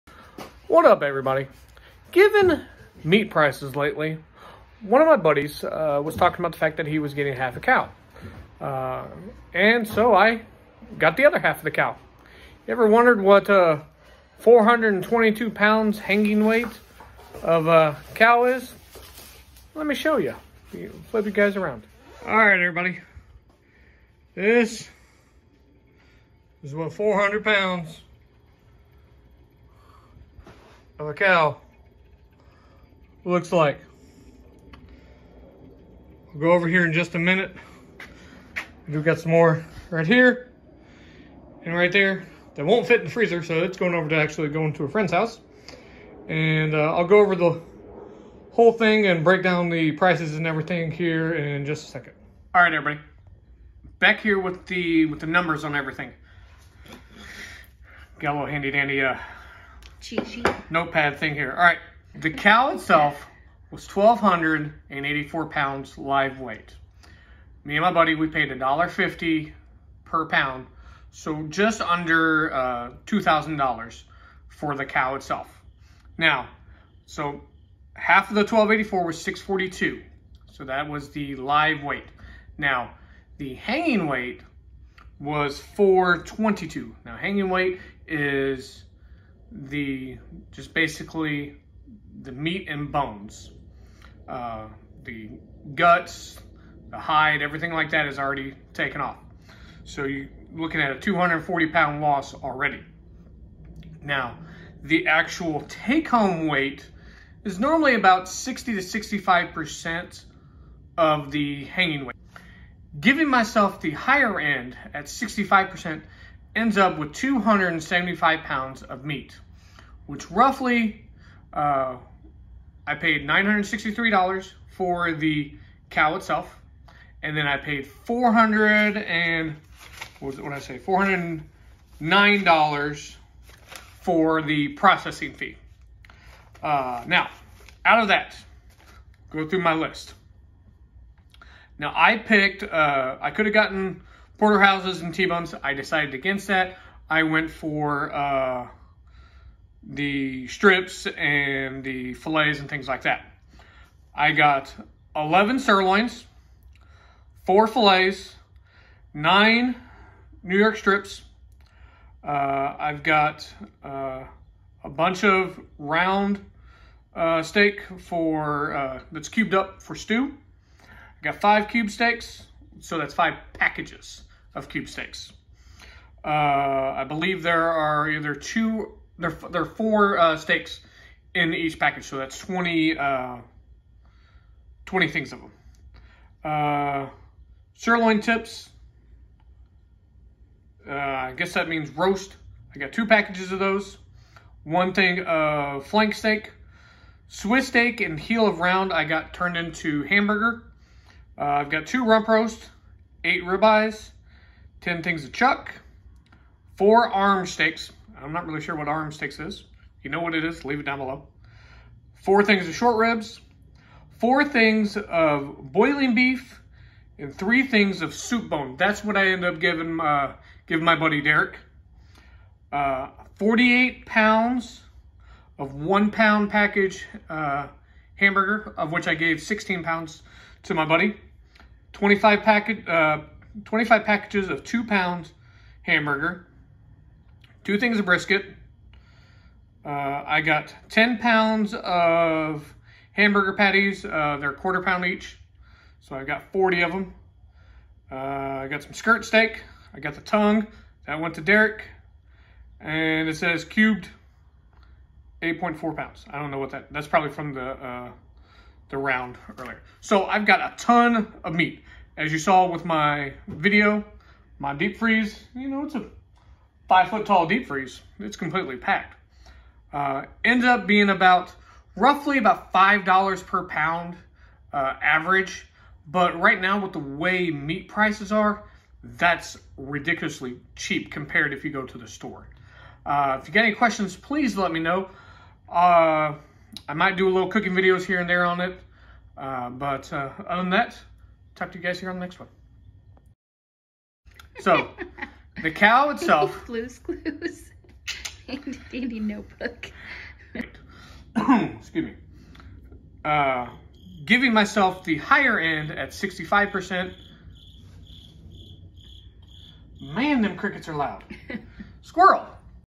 I Bought Half A Cow
Given the cost of meat is soaring right now, I had the opportunity to purchase half a cow to have butchered. Total cost was just under $1400 for around 275 pounds of meat.  This is a great way to stock up for shtf scenarios and support local farmers. Amazon Wish List Items